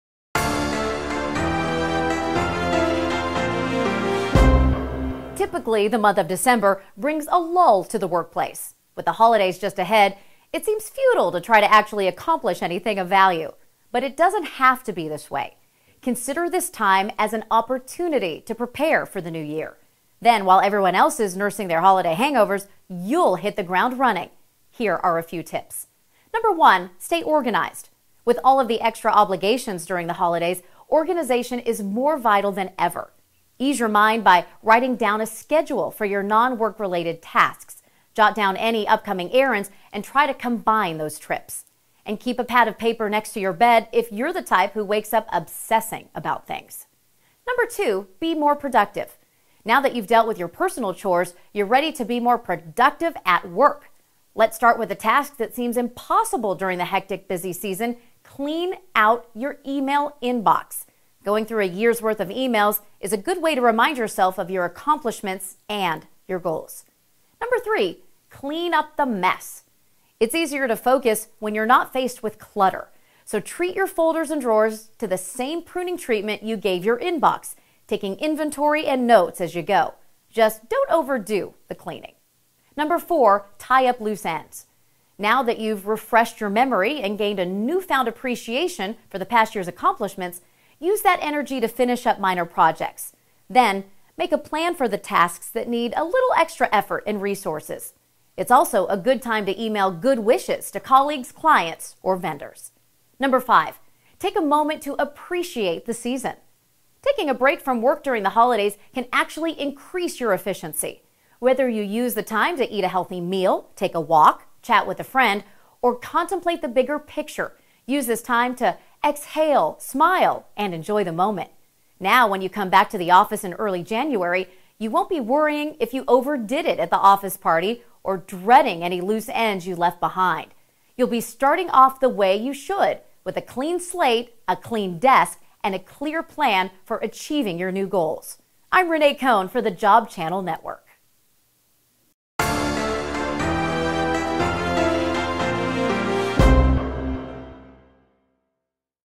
5 Ways to Work Smarter During the Holidays
Job Channel Network's Renee Kohn tells you how to use your work time wisely during the holiday season.

For more news and information about careers and the job market, visit JobChannelNetwok.com.